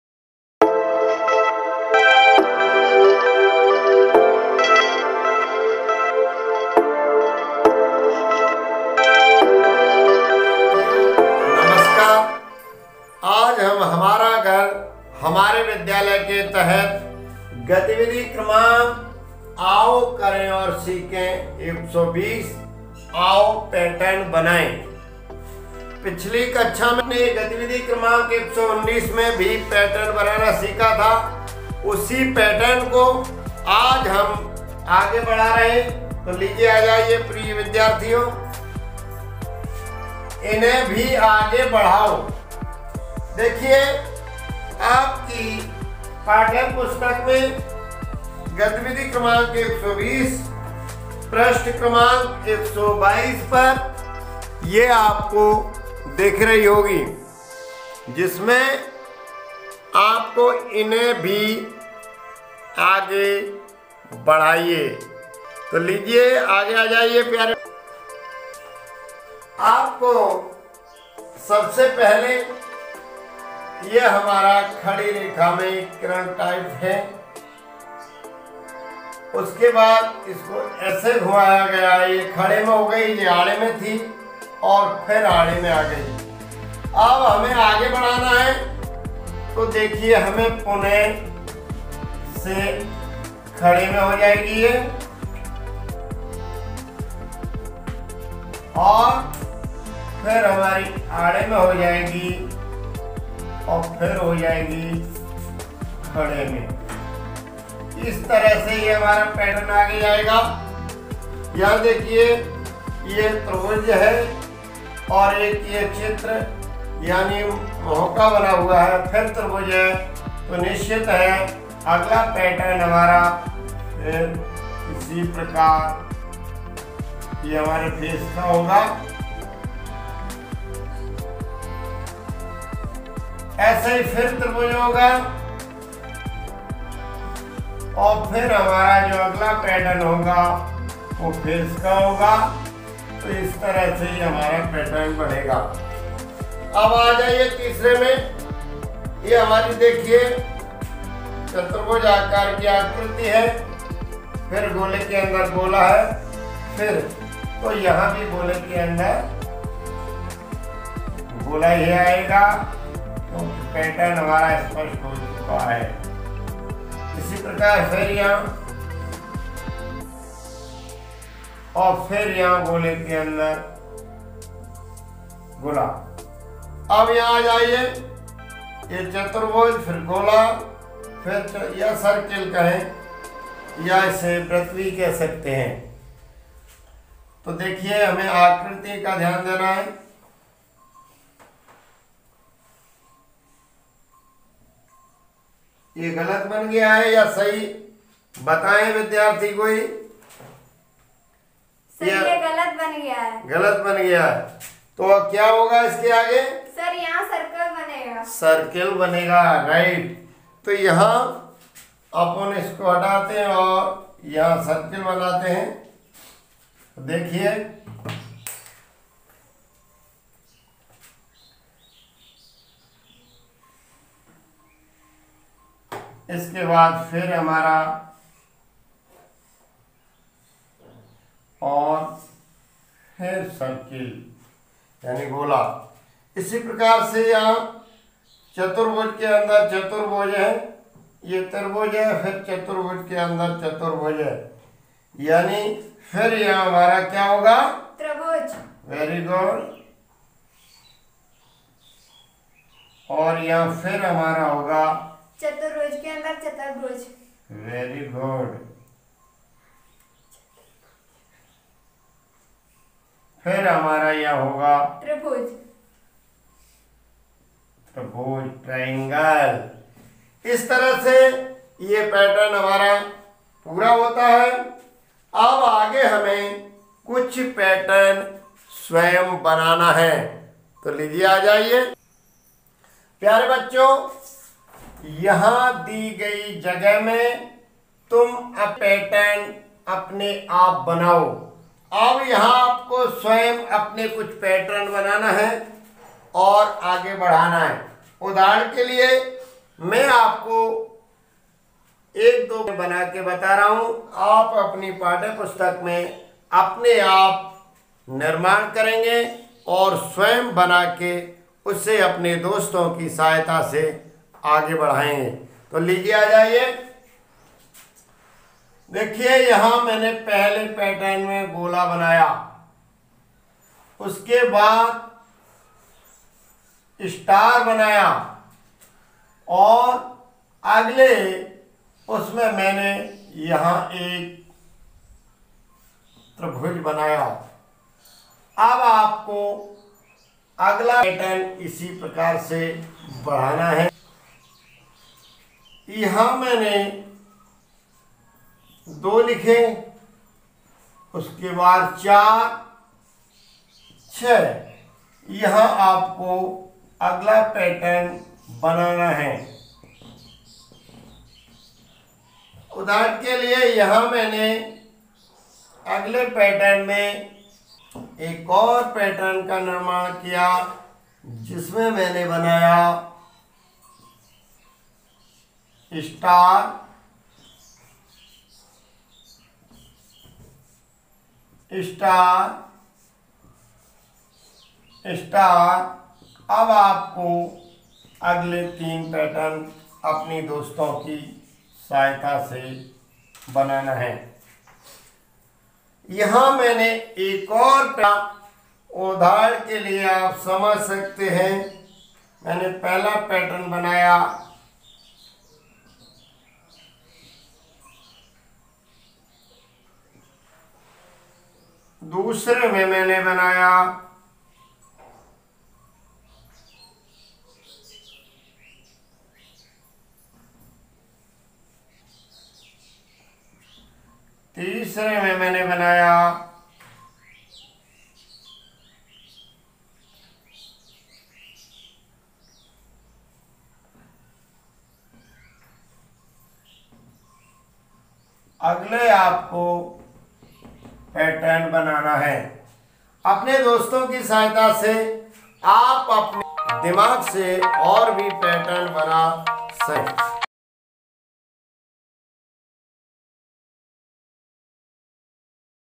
गतिविधि क्रमांक -120, आओ आंगन टप्पू खेलें, पैटर्न की समझन, राकेश सिंह राजपूत
आओ करें और सीखें विषय गणित सीखने का स्तर 3 से 5 पैटर्न के समझ राकेश सिंह राजपूत ,शासकीय माध्यमिक विद्यालय मल्लपुरा, विकासखंड अटेर जिला भिंड मध्य प्रदेश